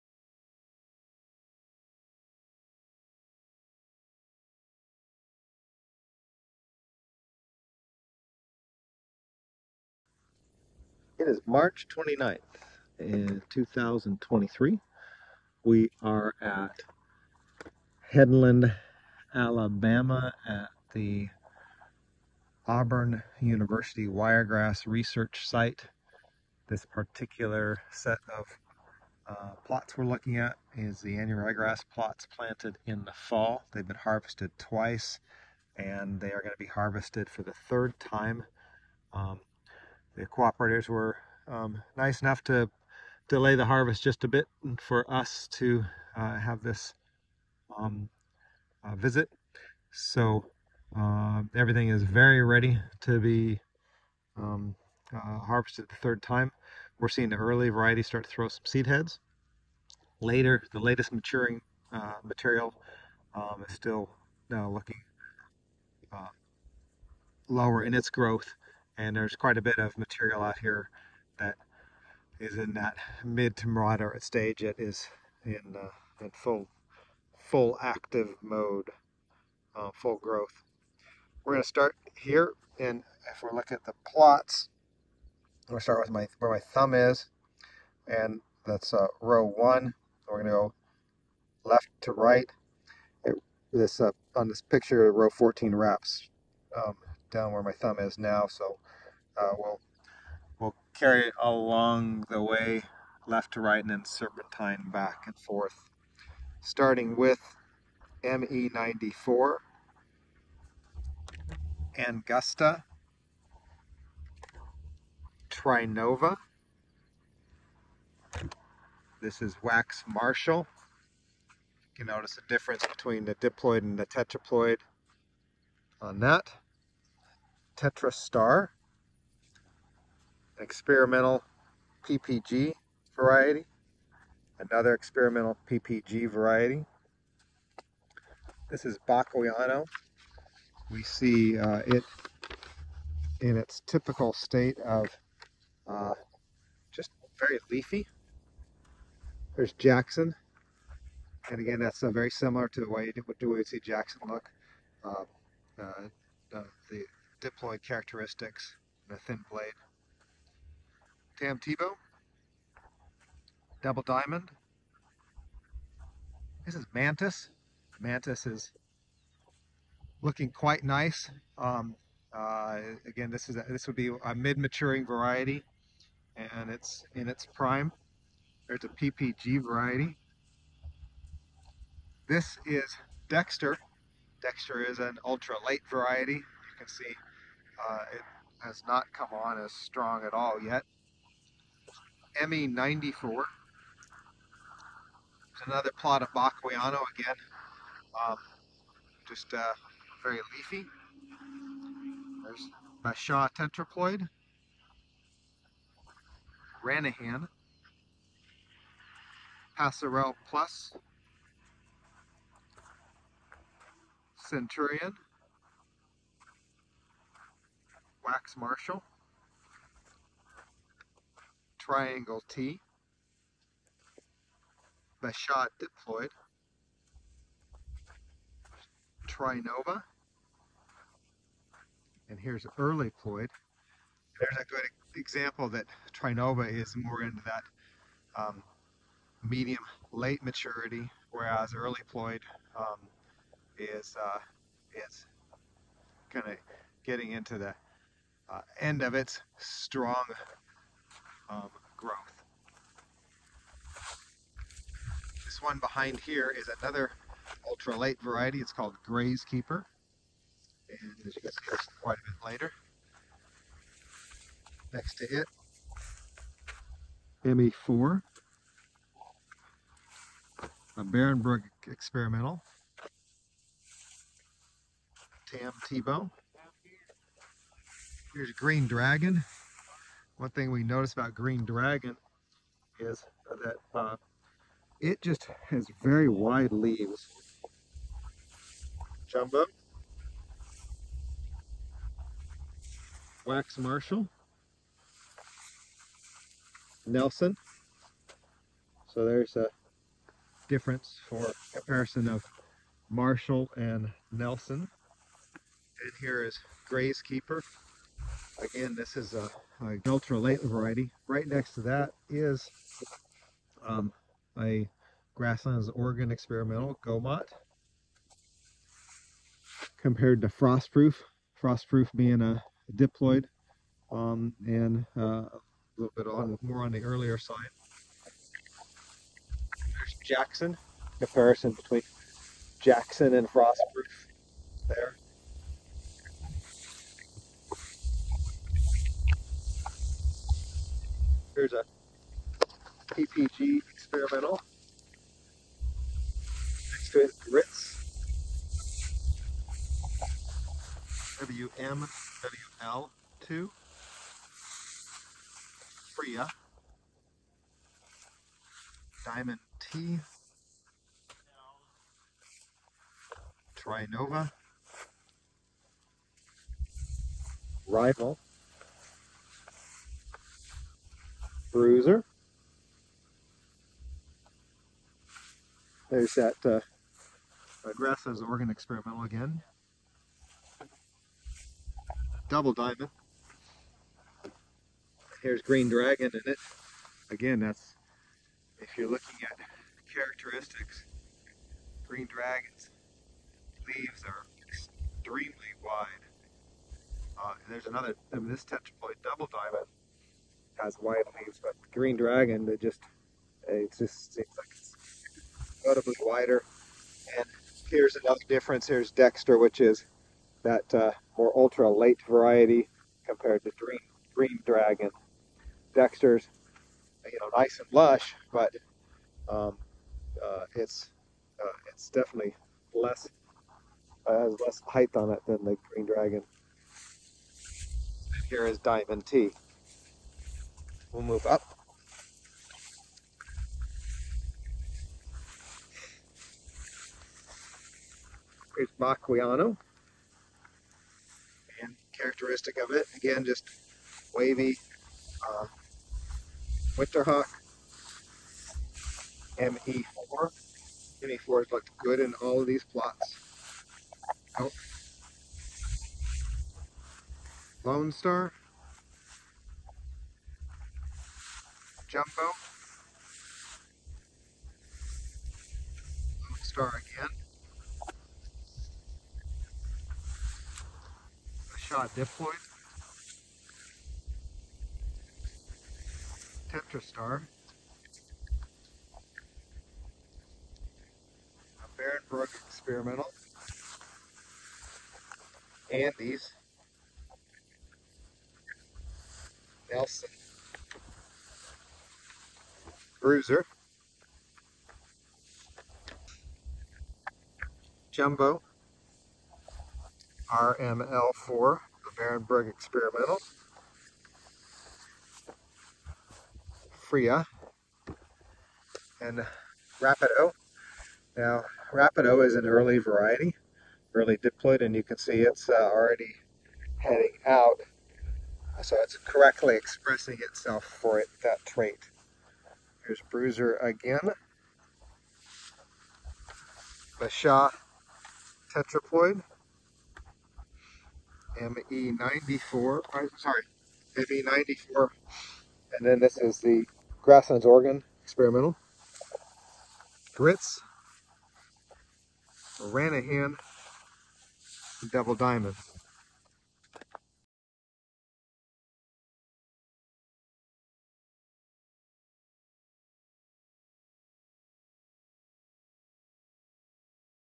2022-23 Auburn University Annual Ryegrass Trial – Headland, AL – Pre-Harvest - Part 1
Join us for Part 1 of a video walk-thru of Auburn University's annual ryegrass trials in Headland, AL.